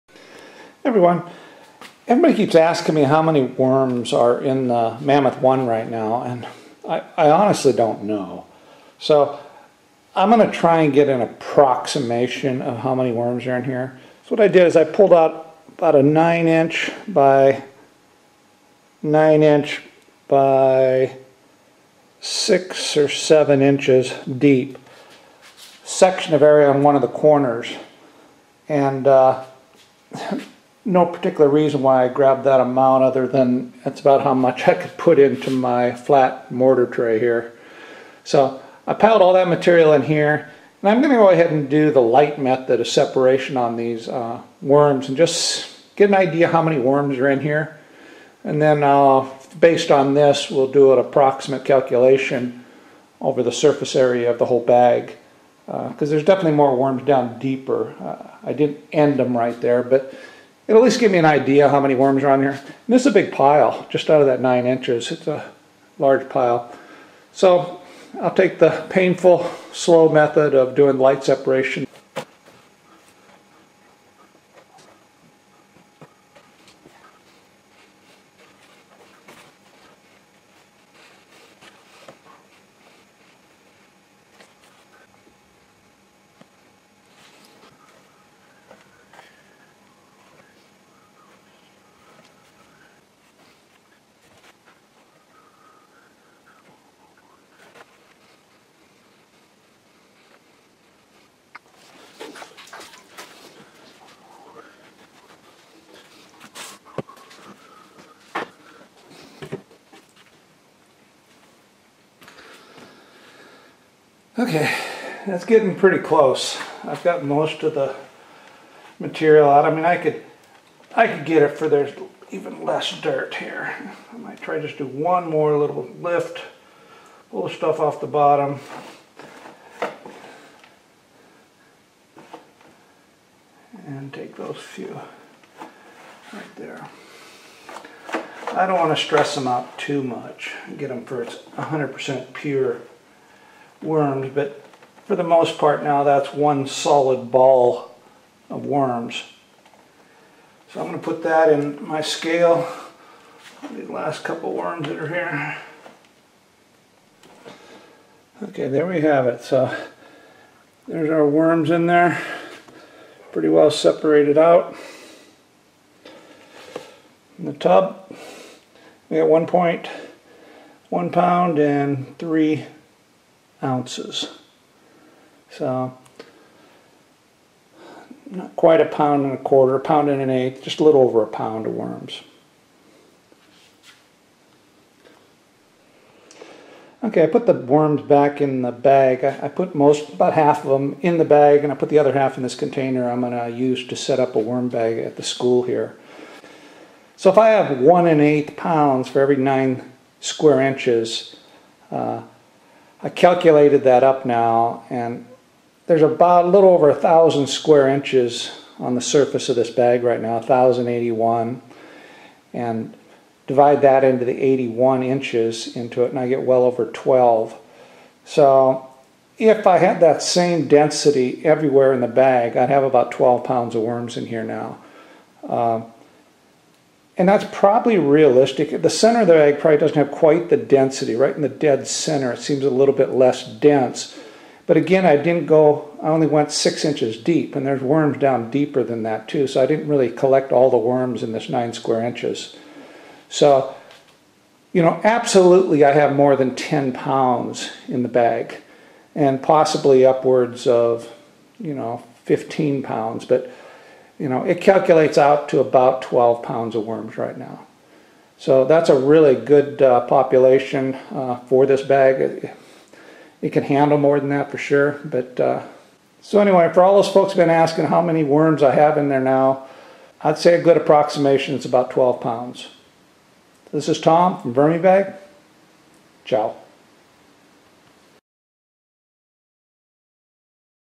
VermiBag Ep 85 How many worms are in the Mammoth One???
I took a sample from the Mammoth One and separated the worms using the light method and weighted the amount of worms in the sample area to get a good approx of the total weight of worms that are currently in the Mammoth One.